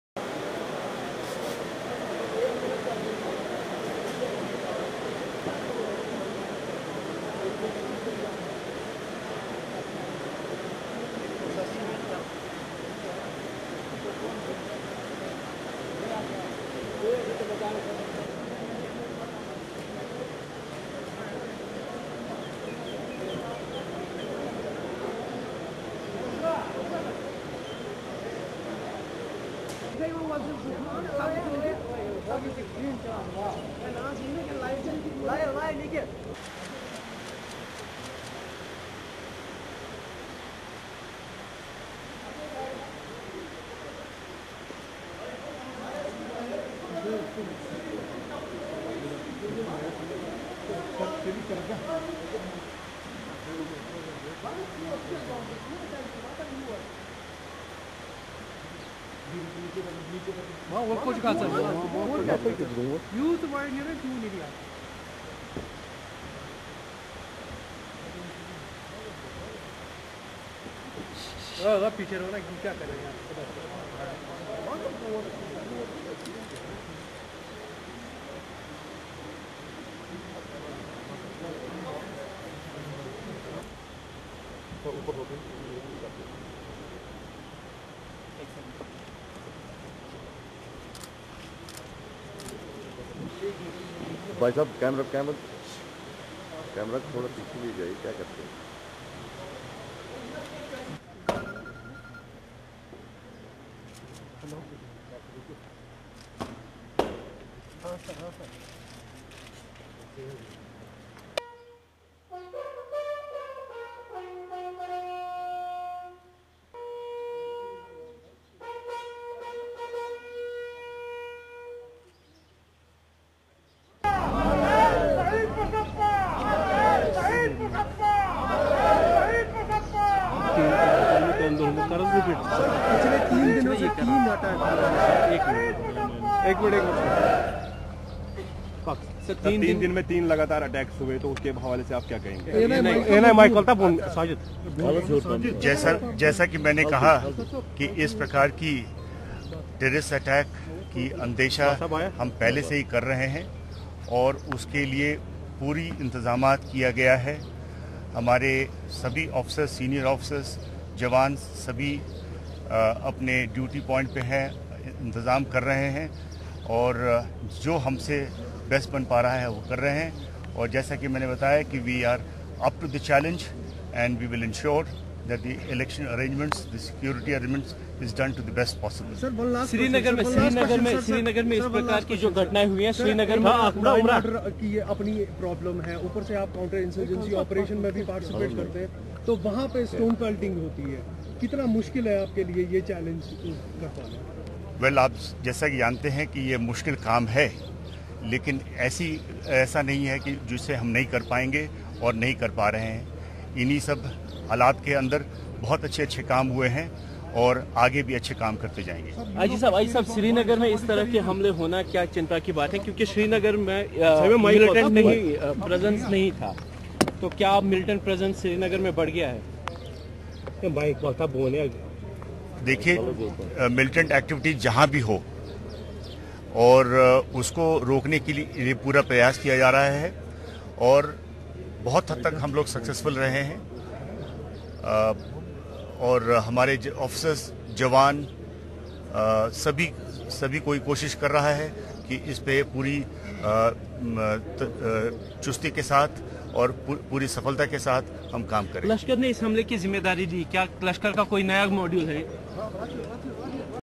Wreath laying ceremony of CRPF man killed in militant attack at Pantha Chowk
Wreath laying ceremony of CRPF Shaheed Basappa Bajantri of Karnatka